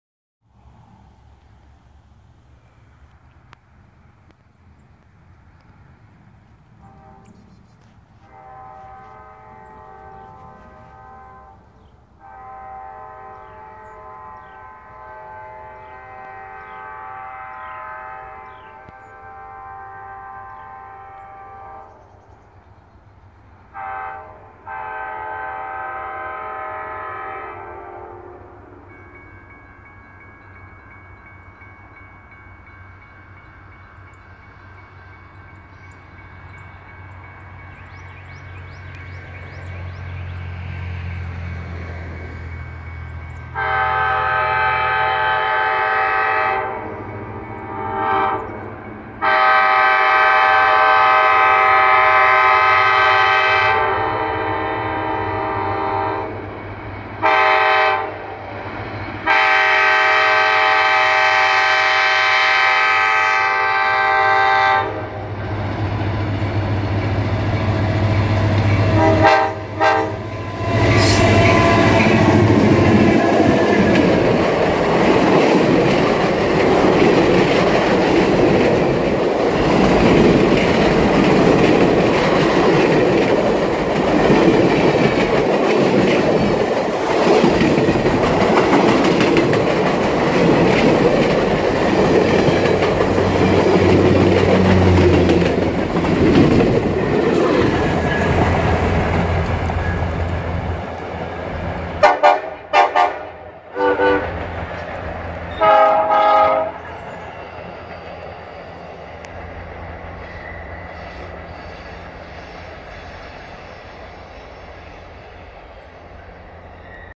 L&N 405
The annual Christmas trip(s) of the Tennessee Central Railroad Museum with L&N GP7 #405 on point east to Lebanon, TN. E8A #6902 gives a triple horn salute at the end